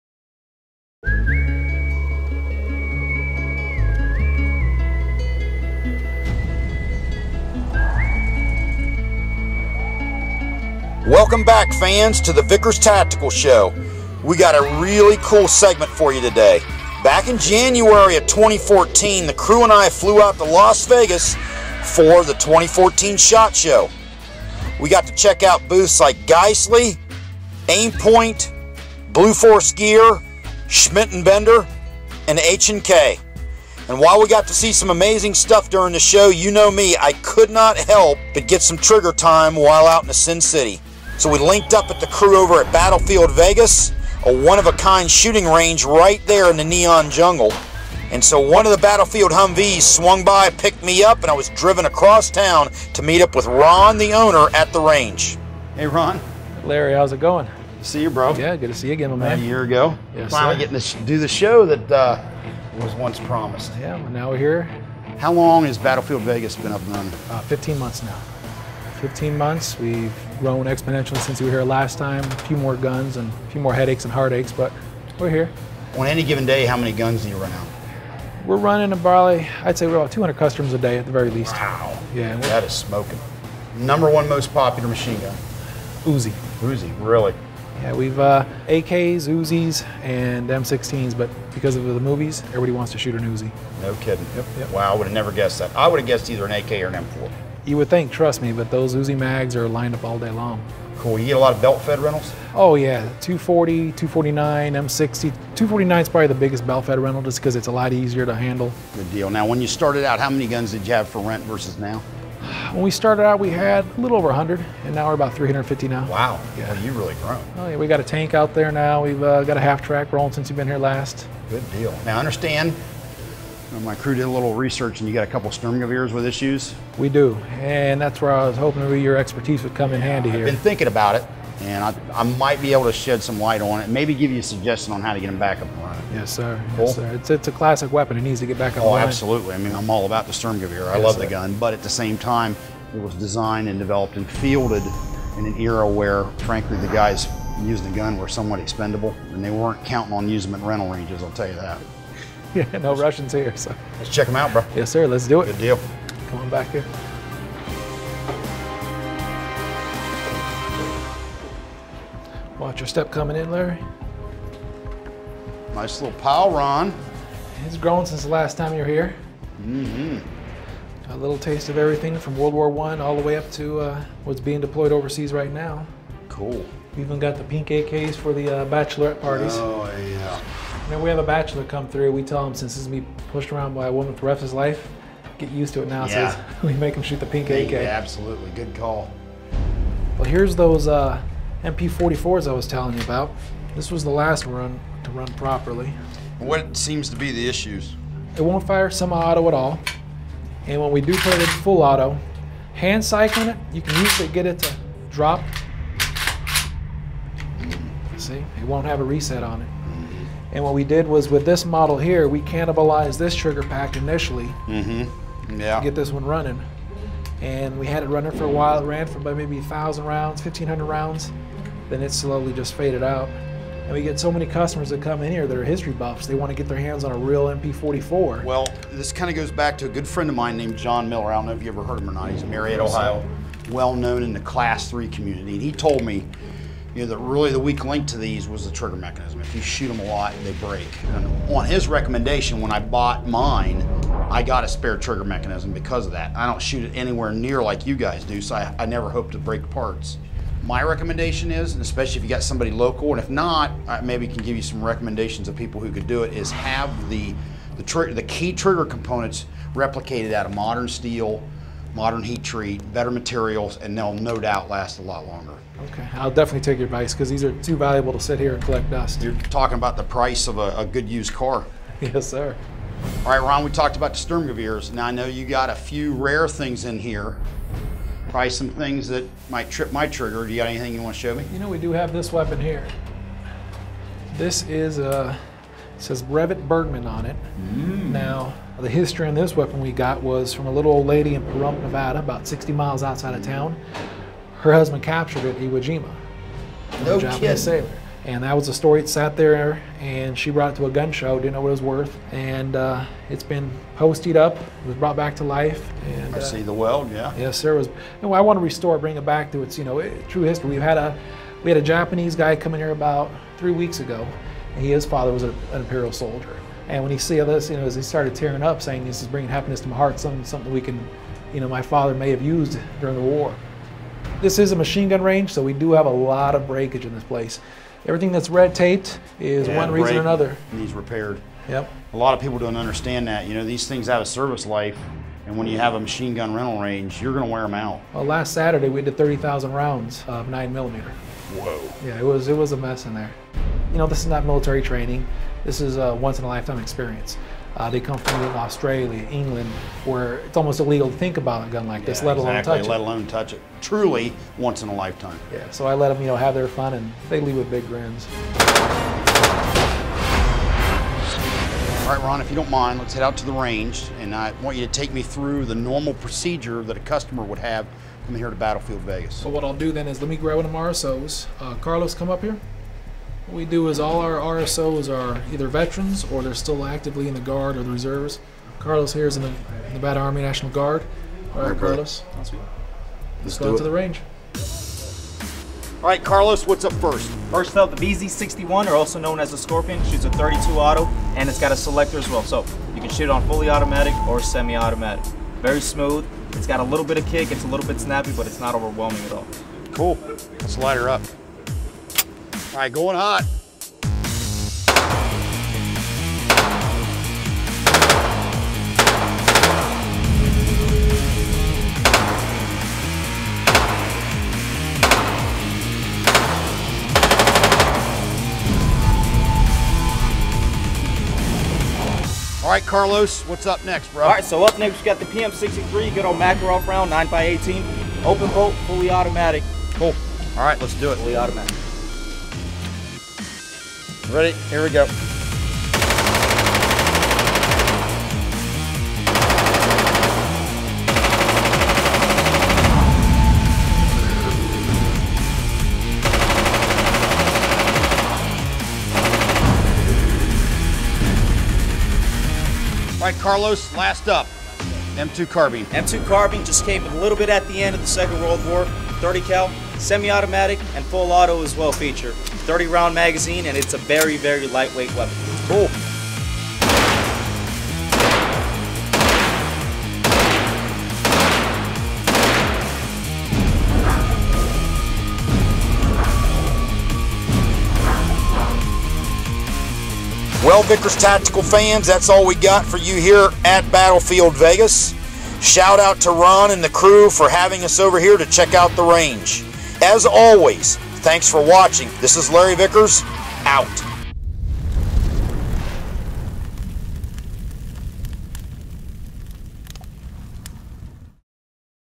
Battlefield Vegas - Shot Show 2014
Larry heads out to Las Vegas to check out Battlefield Vegas...